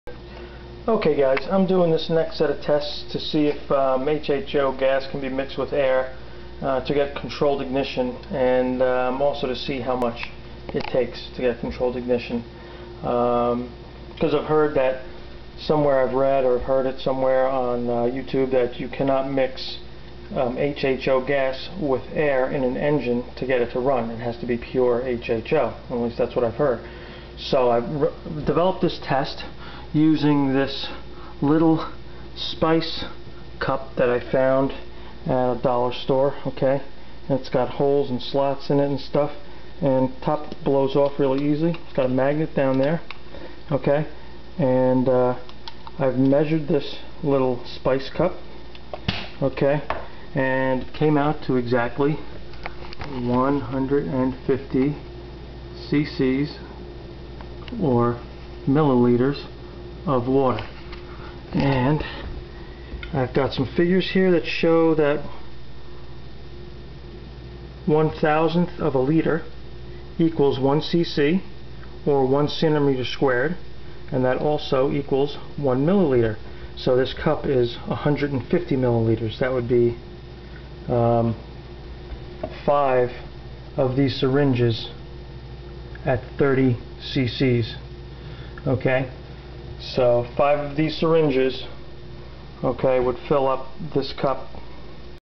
Hydro Lab (My Garage) #30 Mixed Gas
Testing to see how much hho to air will ignite.